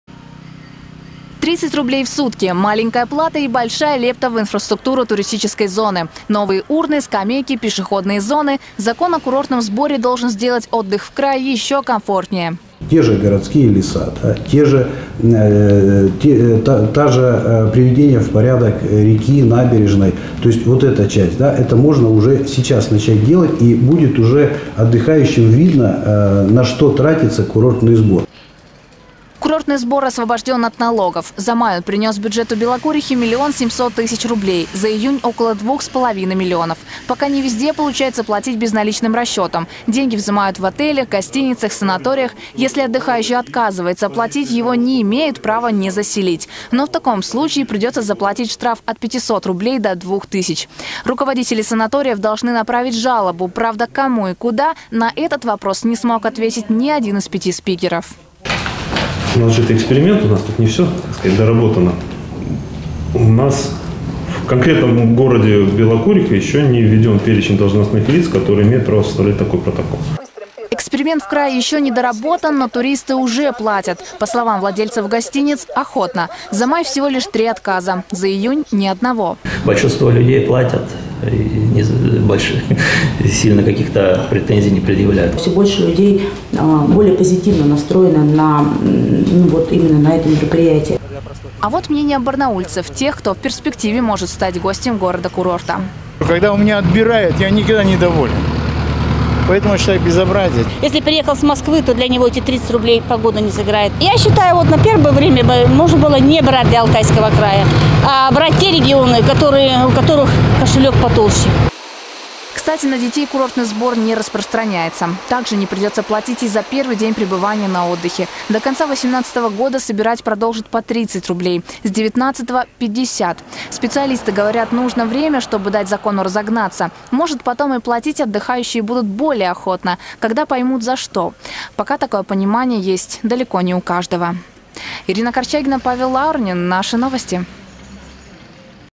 2,5 млн. рублей пойдет на развитие Белокурихи
Собрали эту сумму в рамках курортного сбора за первый месяц  работы эксперимента. Его предварительные итоги сегодня озвучили в рамках пресс-конференции. В управлении по туризму сообщили  - в крае закон ещё адаптируется, но туристы уже охотно вносят ежесуточную плату.